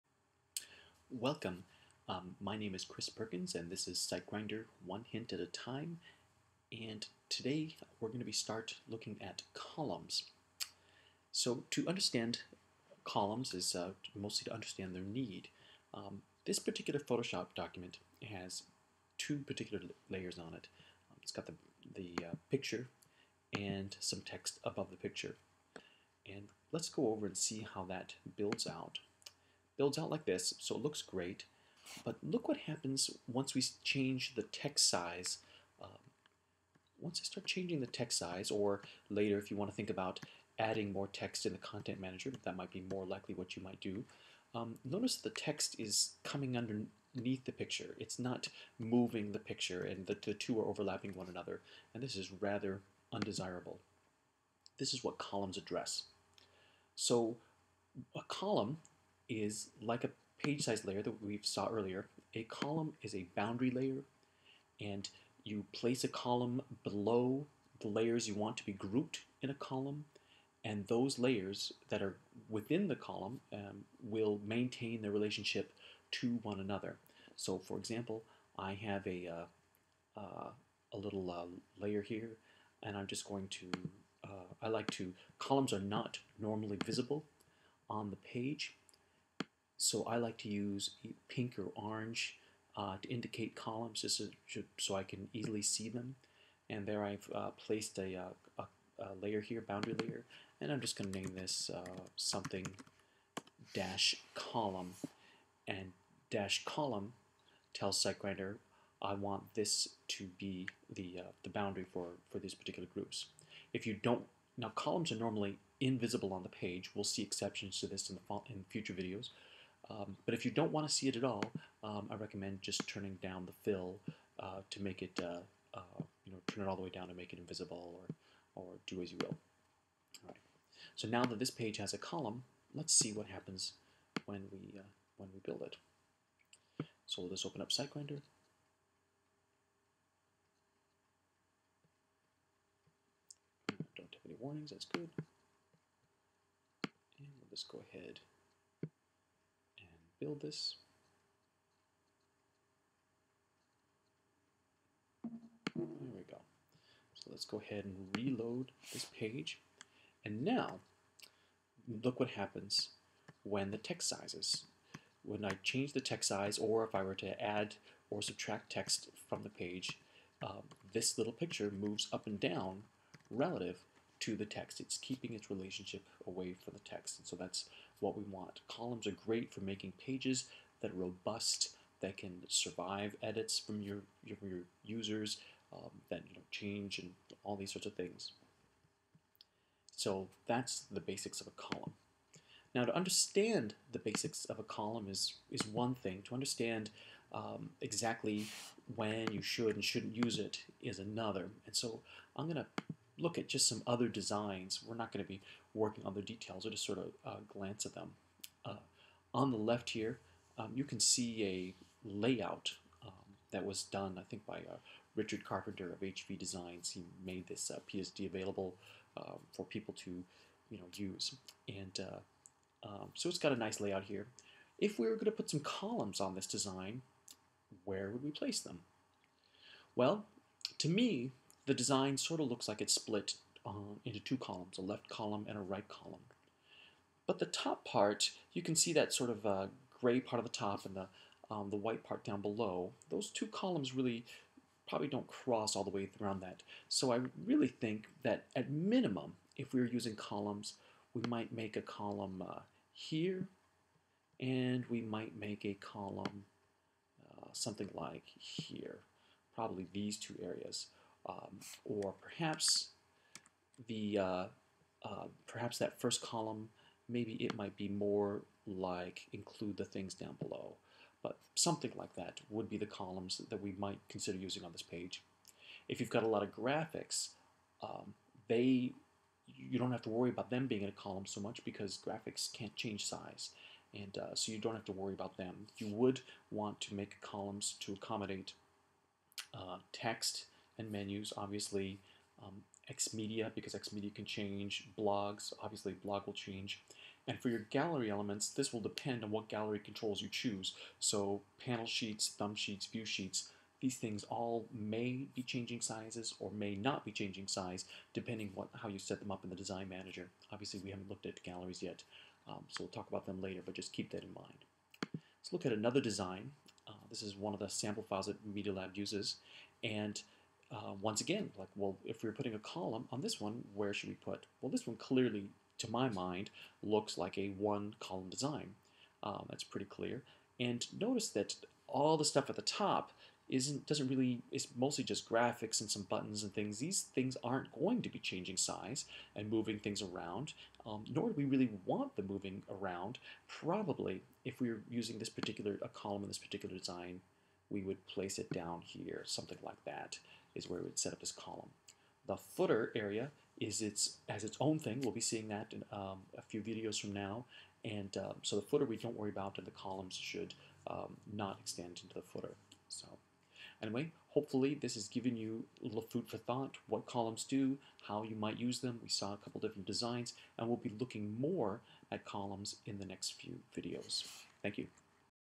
18-Introduction to Columns - Photoshop to HTML Websites with SG3
- This video teaches you how to build flexibility into your design by using the -column hint. Explores some possible layouts that would benefit from using the -column hint.  With SiteGrinder you can create a website in Photoshop and then let SiteGrinder convert your web design from PSD to HTML.